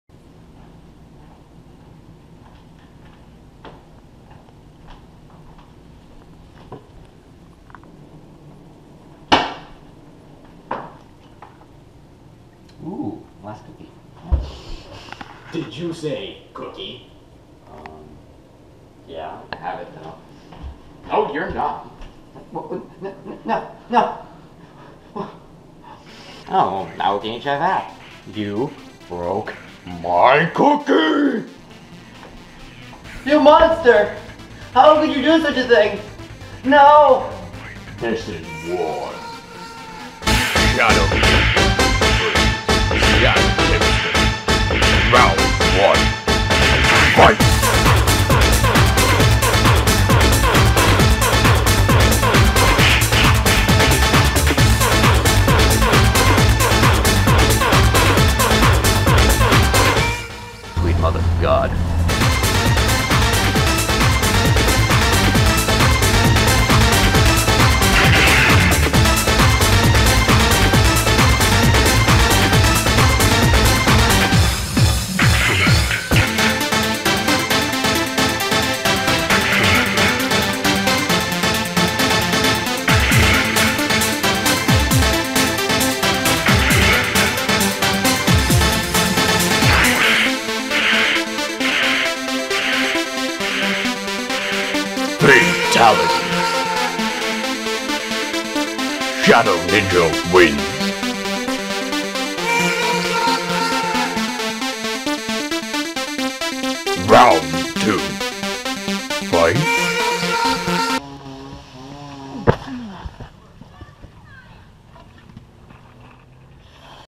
Mortal Cookie Kombat
When two friends desire the last cookie in the cookie jar but due to incompetence, break the object of value and engage in a fight to the death.